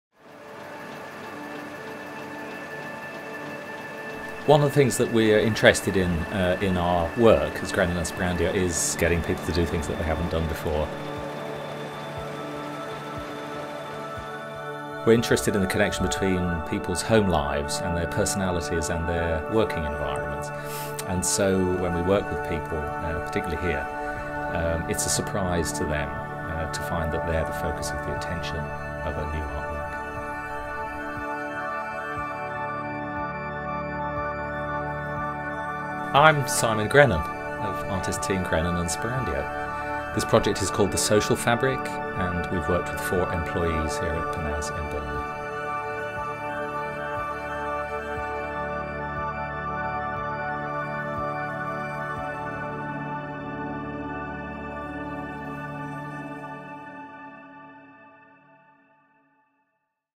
ART IN MANUFACTURING 2017 // The Social Fabric - Grennan & Sperandio and Panaz
The Social Fabric used Panaz’s archive to investigate the life experiences of four employees of the Burnley-based manufacturer, threading the fabric of their lives into bespoke textiles and wallpapers.

From the finance office to the shop floor, four employees of Panaz had their lives, loves and work experiences turned into textile artworks for their homes. The duo were fascinated by the connection between homelife, personality and work environments, and aimed to get the employees of the fabrics firm doing things they hadn’t done before.

During their residency they surprised Glenn, Peter, Martine and Anita by making them the centre of their work, holding intensive design sessions in their kitchens, living rooms and workplaces to create fabric patterns featuring coy carp, Whitesnake, Tibetan mandalas and King Charles Spaniels.

Installed in a town centre coffee shop the resultant wallpaper, curtains, sofa and bed runner inconspicuously evoke elements of each worker’s personality and personal history with patterns as idiosyncratic as the four collaborators themselves. The artists’ creations span the space between worklife and homelife; the fabric of the employees’ lives threaded through the fabric of their factory.

Filmed by Wash Design, commissioned by Creative Lancashire